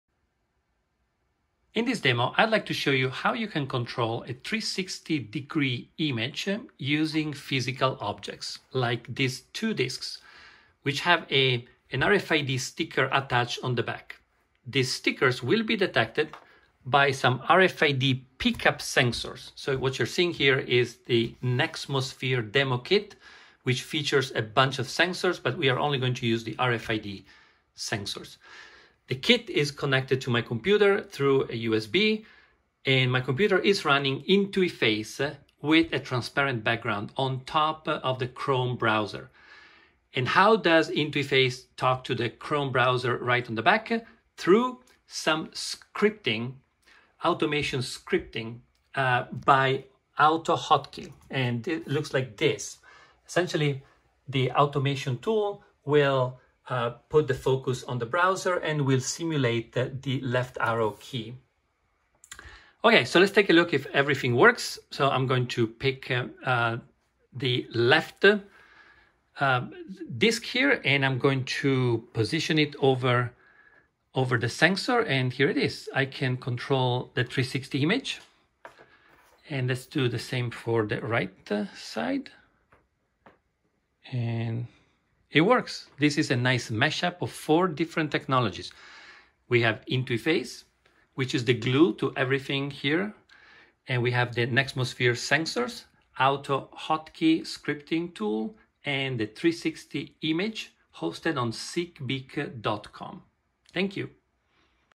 How to control 360 media using Nexmosphere RFID sensors, Intuiface and AutoHotKey
- In this demo we feature the use of RFID sensors to control 360 degree media. This mash-up makes use of four different technologies

The goal is to demonstrate how to interact with immersive content through the manipulation of physical objects.

Created by Paolo Tosolini